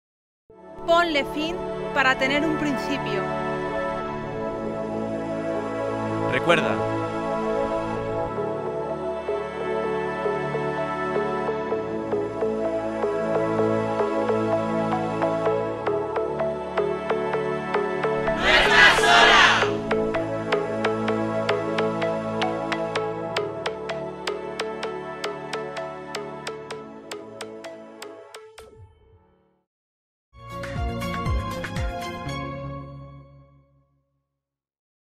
Día Internacional de la Eliminación de la Violencia Contra la Mujer en la Universidad Isabel I
La Universidad Isabel I se suma al Día Internaiconal de la Eliminación de la Violencia Contra la Mujer con un vídeo en el que participan integrantes de su equipo humano que desarrollan su labor profesional en las áreas de Rectorado, Personal Docente e Investigador y Personal de Administración y Servicios.